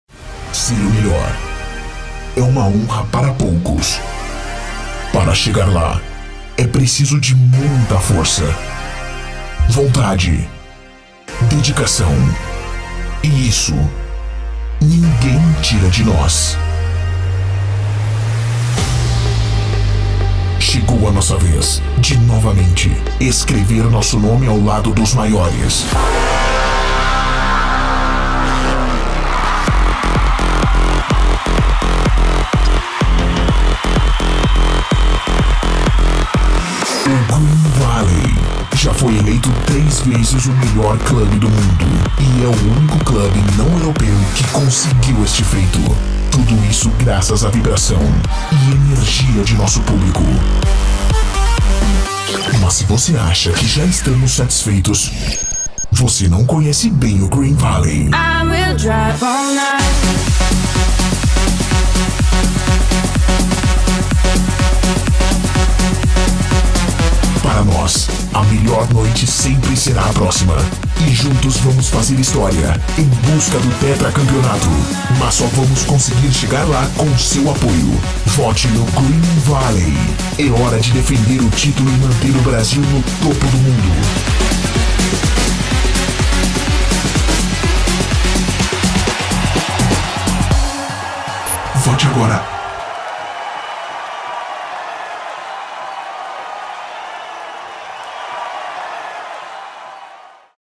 Green Valley • Votação DJ MAG
O Green Valley, líder da última edição, parte rumo ao "tetra" na campanha que agora entra na reta final!

Do Brasil para o mundo, fizemos a diferença ao sermos o único club fora da Europa com esta premiação e por três vezes (2013, 2015 e 2018).

For the third time in its history, after 2013 and 2015, Green Valley, the 2018 ranking leader, has made it all the way to the top of this list, the most prestigious international club ranking and wants to repeat it again!

Your vote is very important for Green Valley to keep its prestigious podium position.